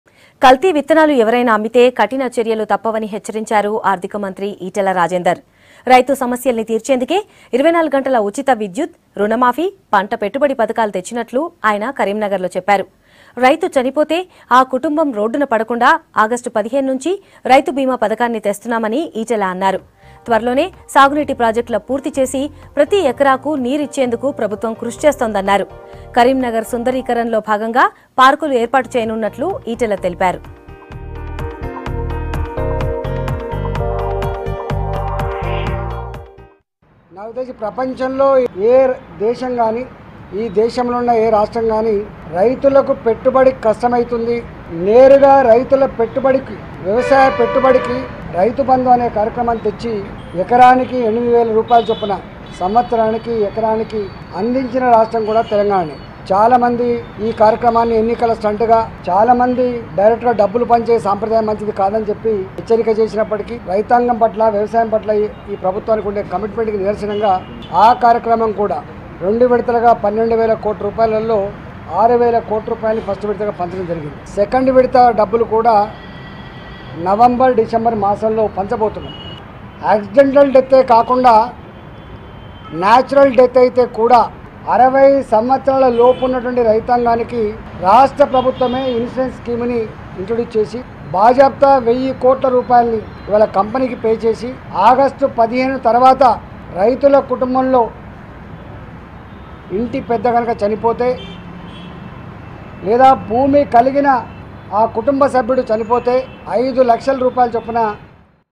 Finance Minister Etela Rajender: Government Will Start Rythu Bheema Scheme On August 15th | V6 News
Finance Minister Etela Rajender: Government Will Start Rythu Bheema Scheme On August 15th 2018.